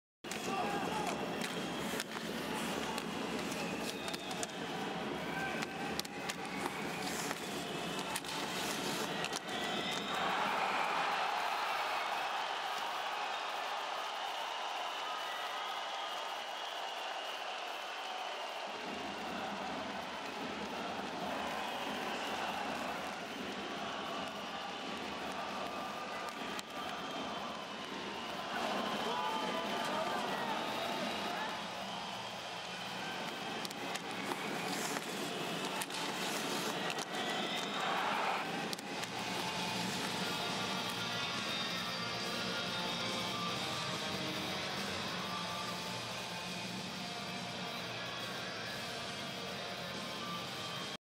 Konstantin Okulov beats Yeryomenko short-side just inside the post
Sibir's Okulov wins the battle in the corner, then moves to the net and makes an accurate shot inside the post.